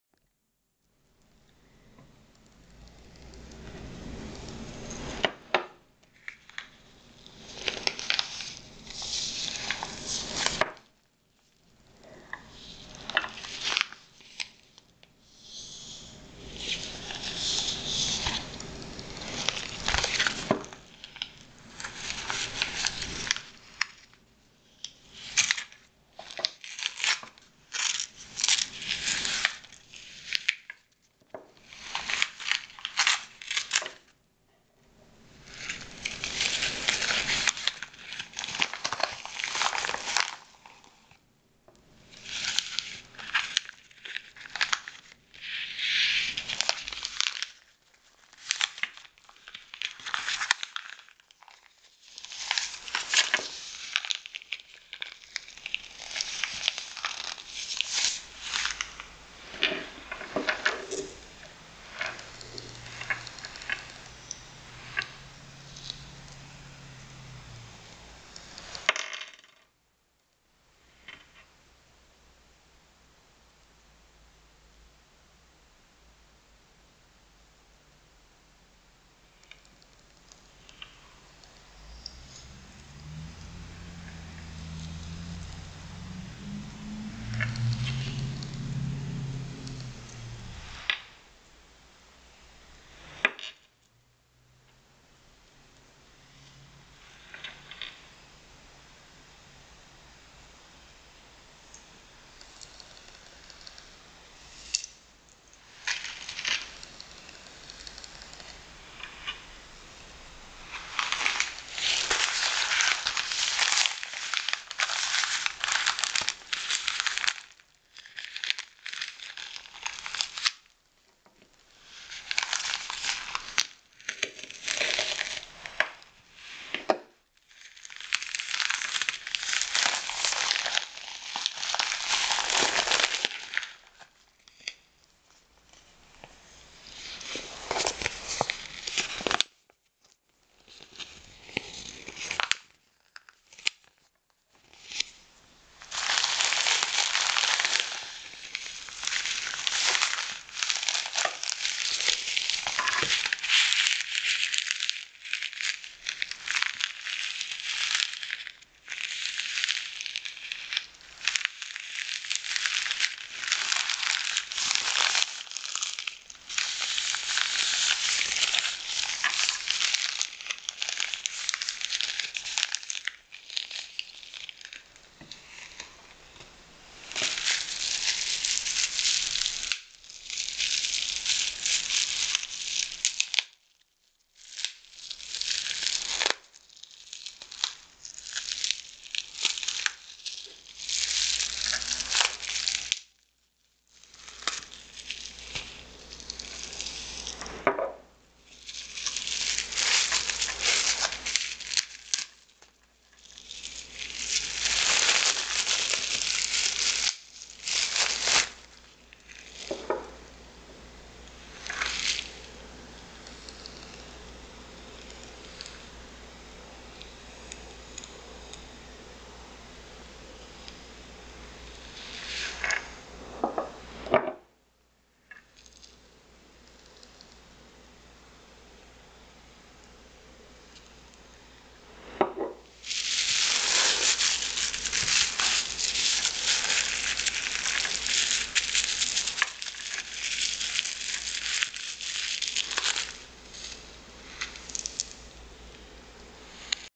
ASMR: Silent Unpacking (pendulum and polish)
Another silent unpacking for you all. The polish is Layla Black as Ebony (chrome) and also Ludurana Emocianante. It smells like violets!